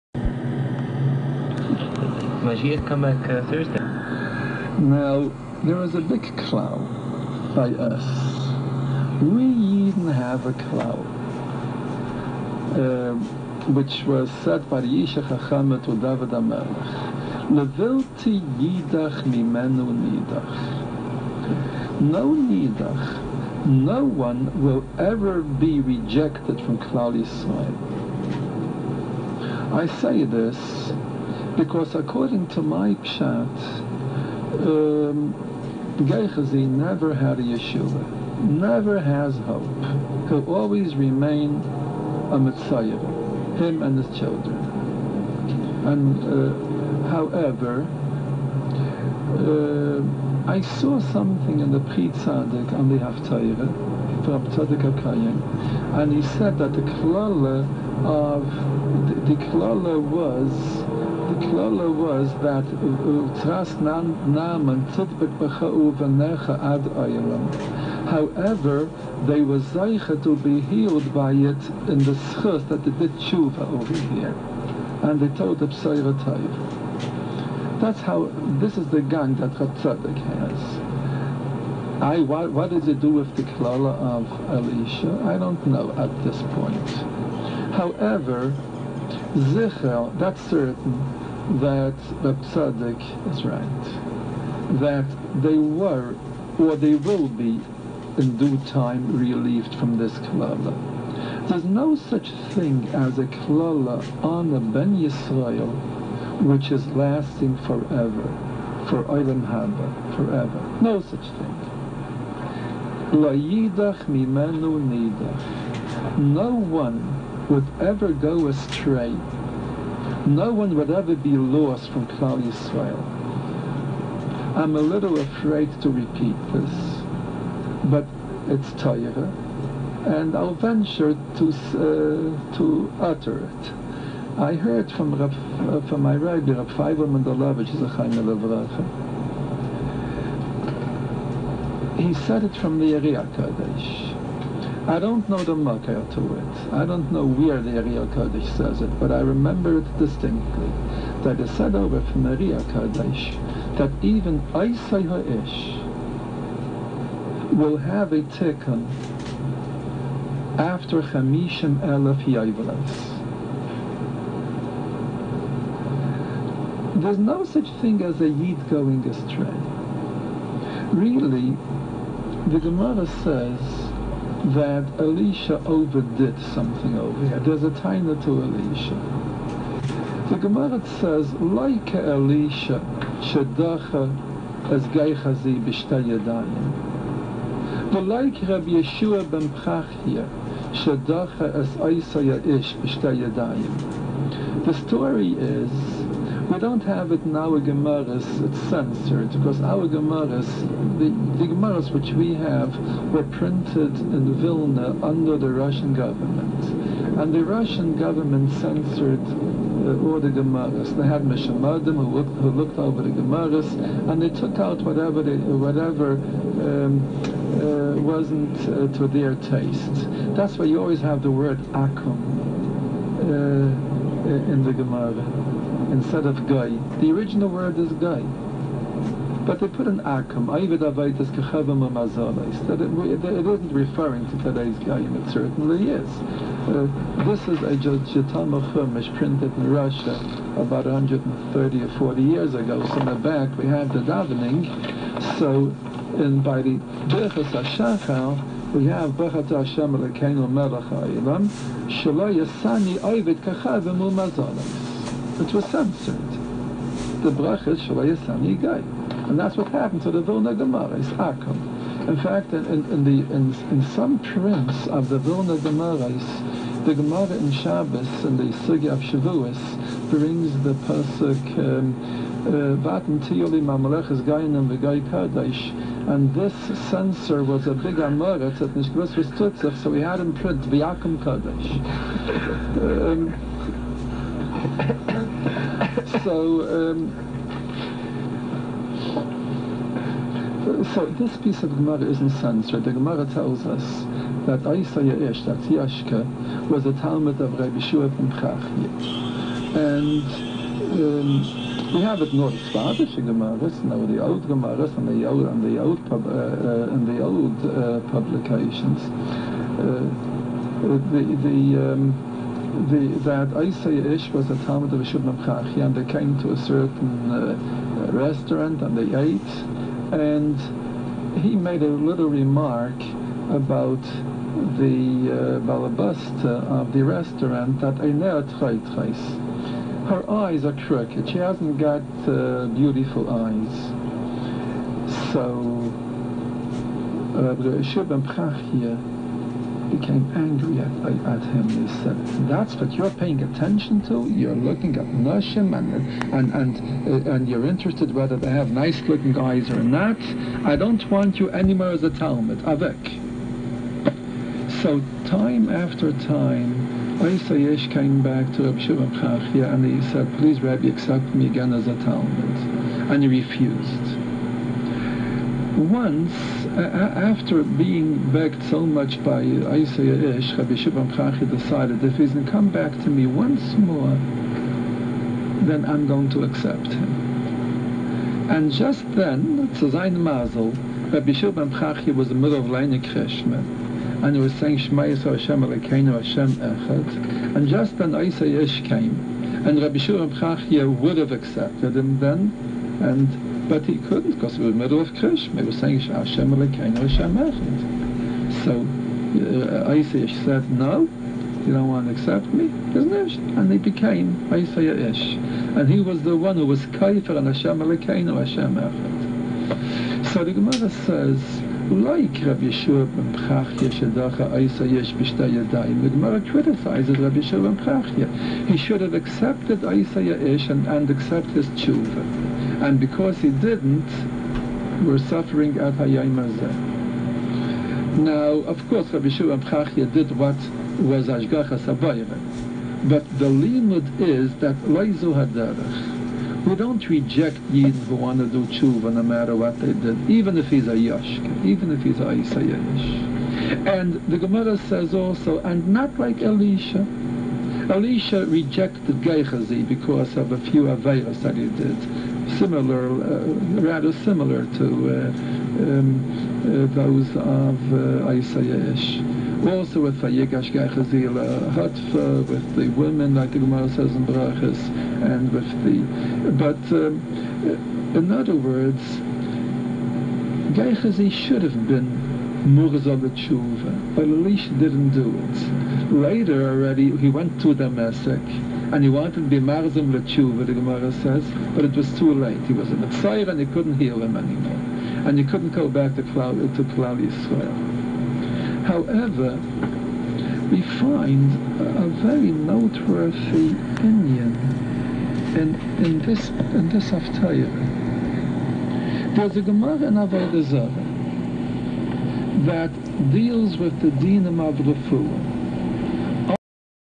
Rav moshe wolfson: why yushka went off the right way and will he ever return
From a drasha of the haftara מצורע 1989 tazria